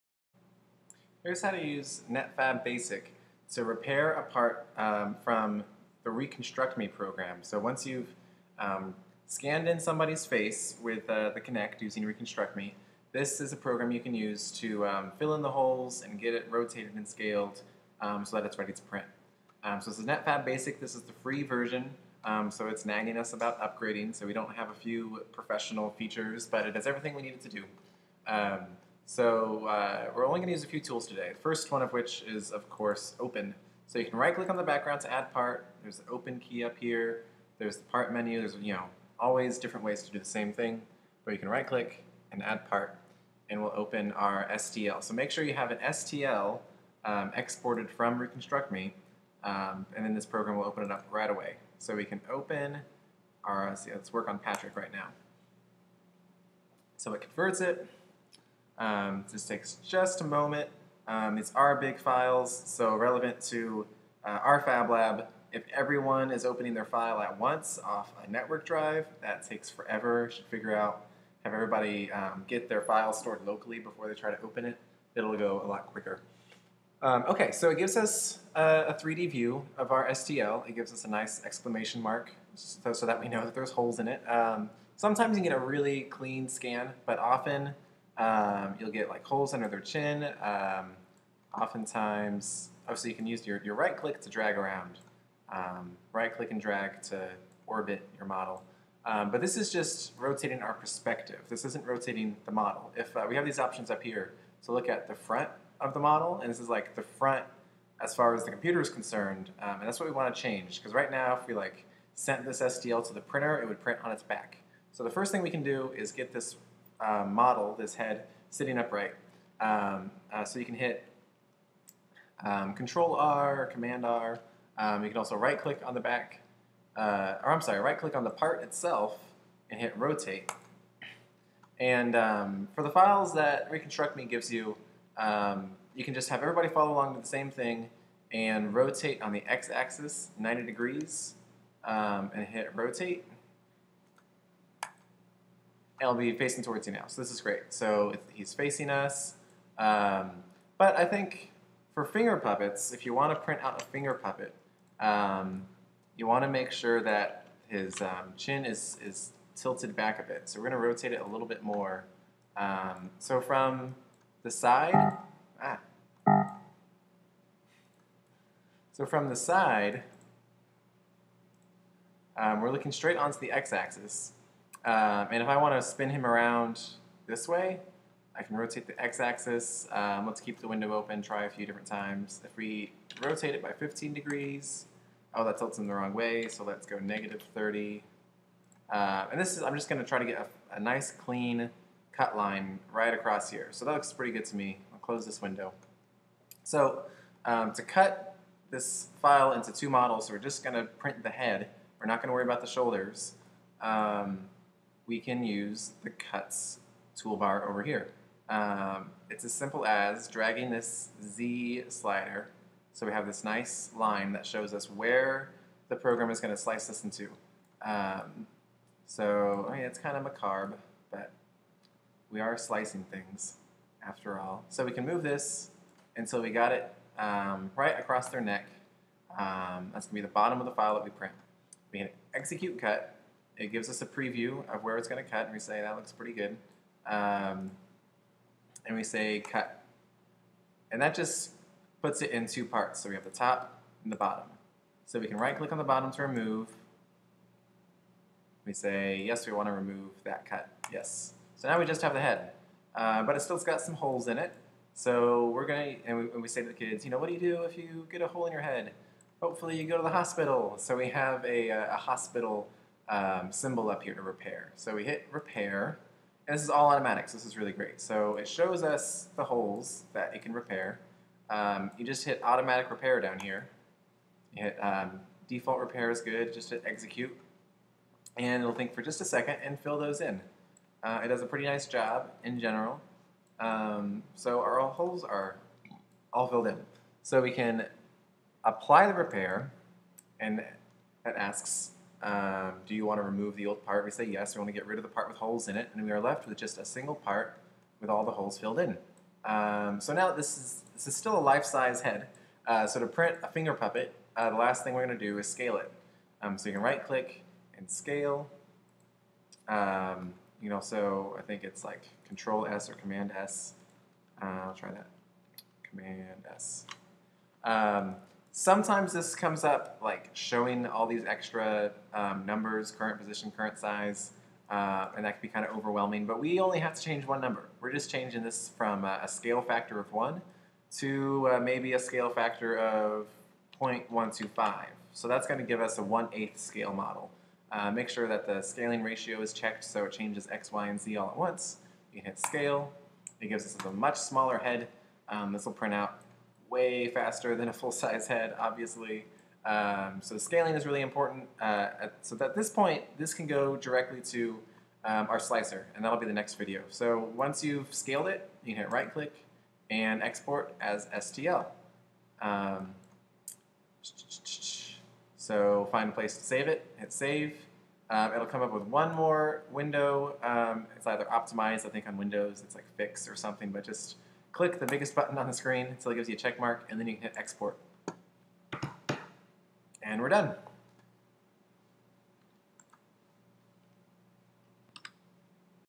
Tutorial: Preparing a fingerpuppet from ReconstructMe with NetFabb.
This is the process we learned at the Wanger Family Fab Lab to rotate, cut, and repair a STL file exported from RecontructMe. This process is straight forward enough that you can have a whole class follow along and repair their own heads so that they're ready to 3D print.